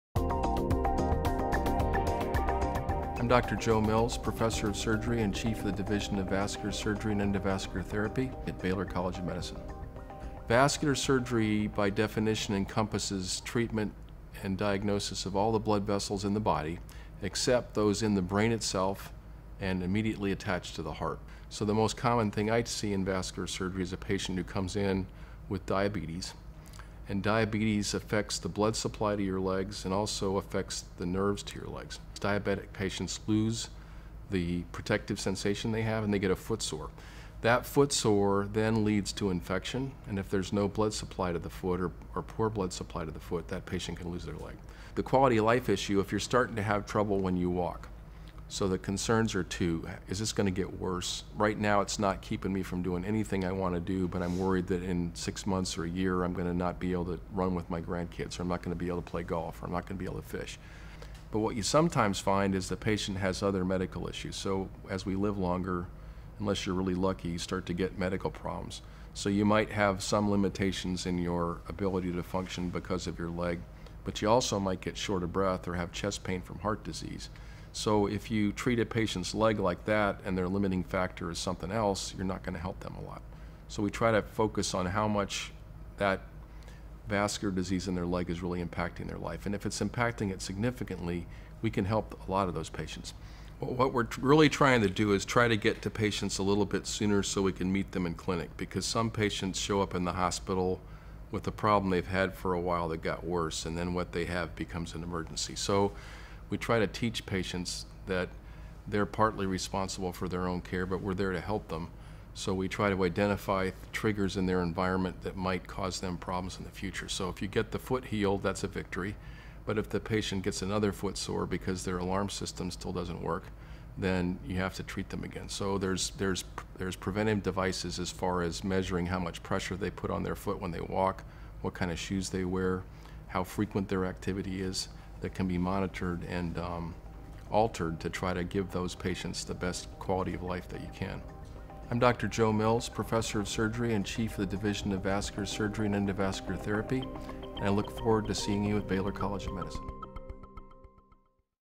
What is Vascular Surgery? Dr. Joseph L. Mills Answers
Dr. Joseph L. Mills discusses the relationship between diabetes and vascular disease.